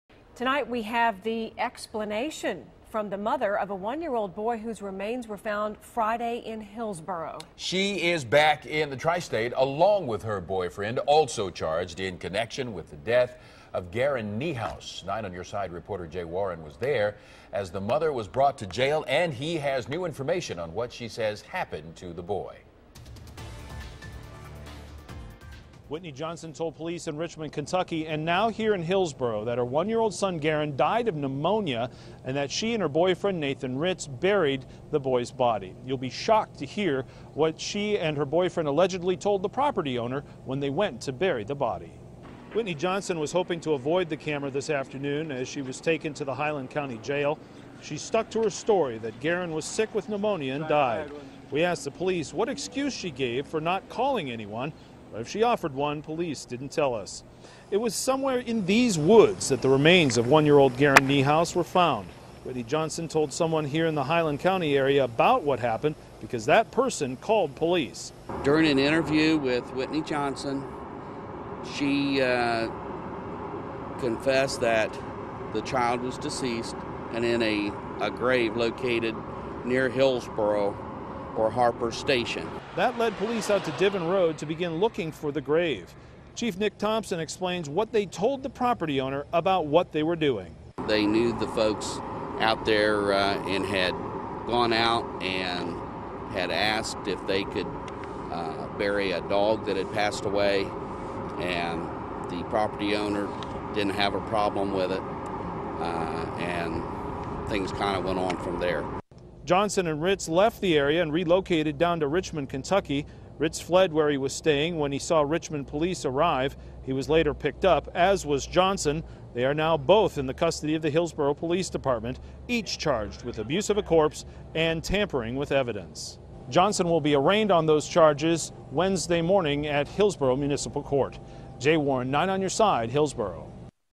Mother: Infant found dead had pneumonia
The mother of a 1-year-old whose remains were found in Hillsboro on Friday said the infant died of pneumonia.